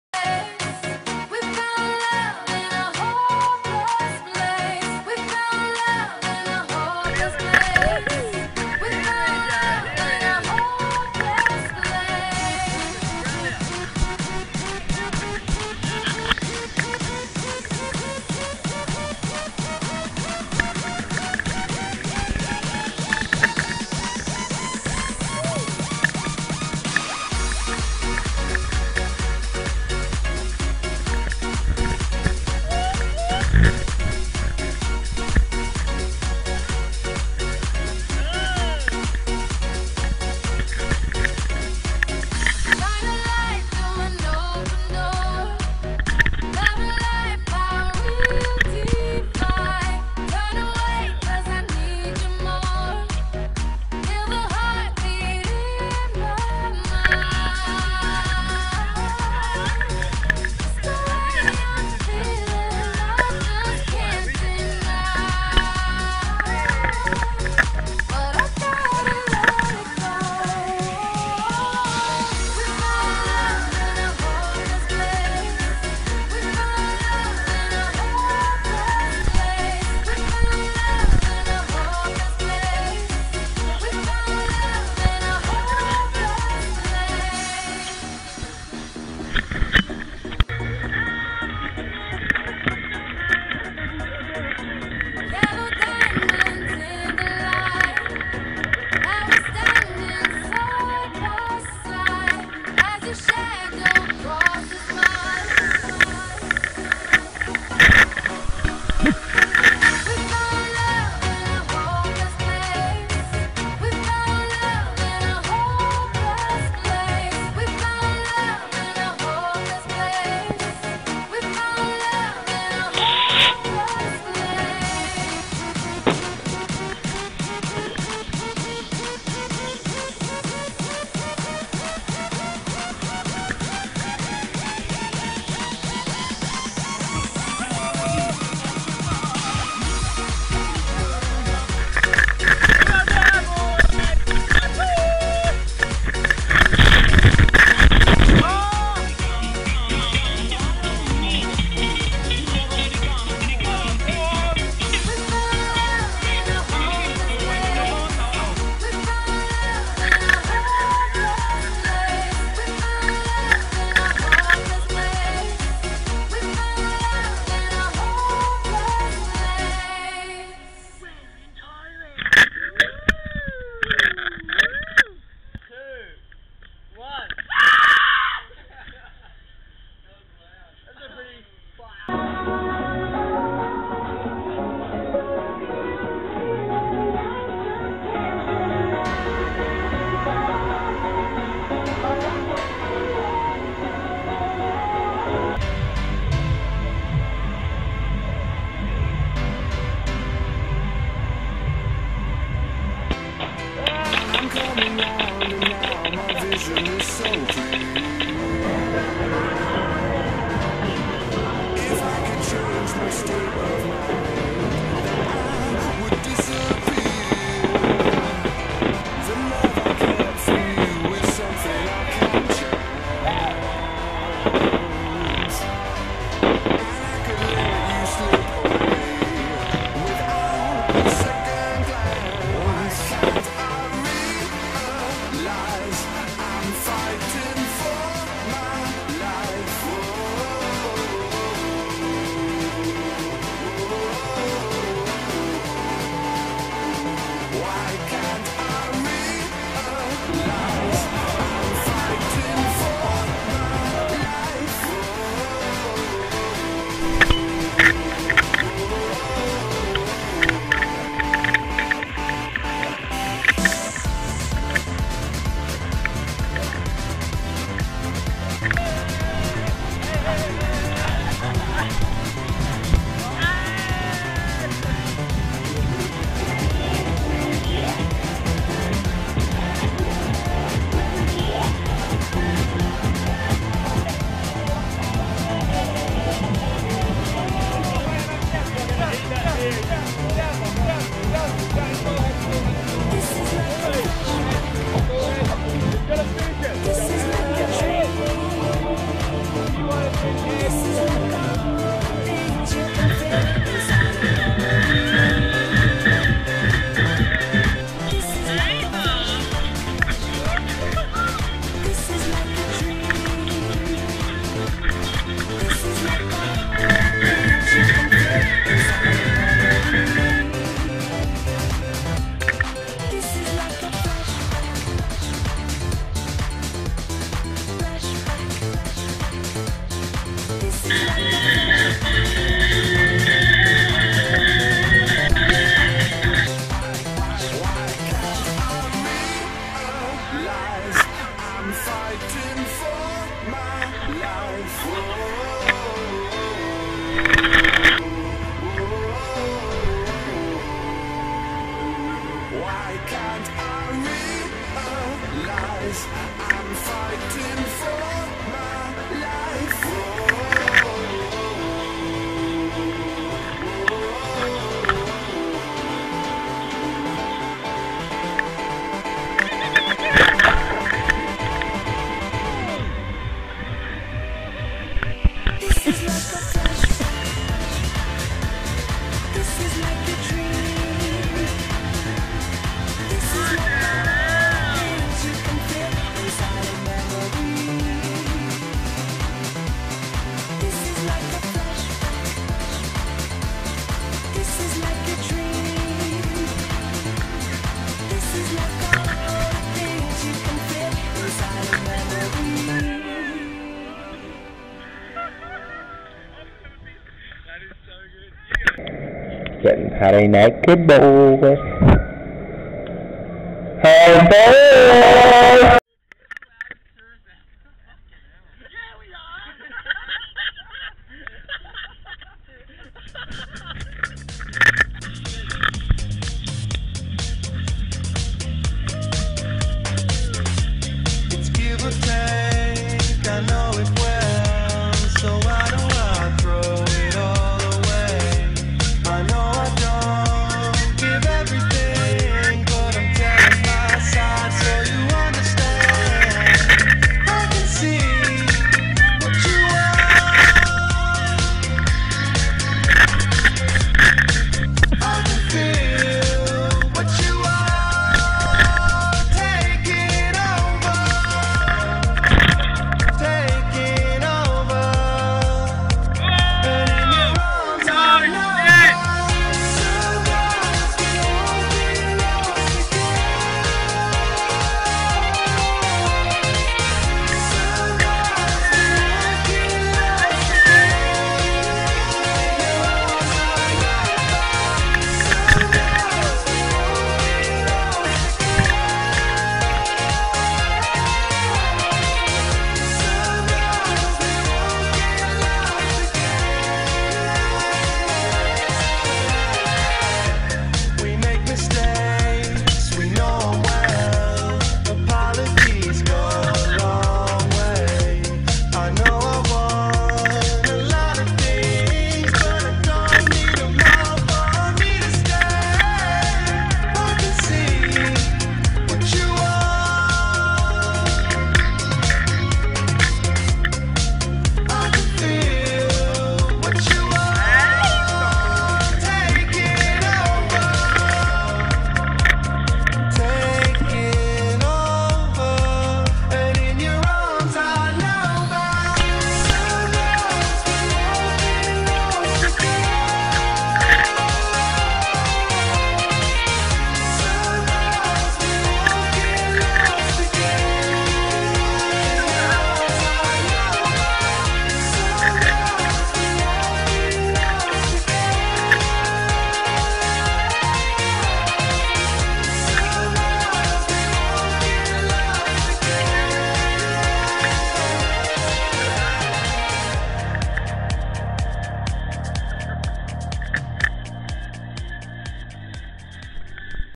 THE BOYTHS - Thailand & Laos 2011/12
The greatest fun five young men can have. A holiday to remember.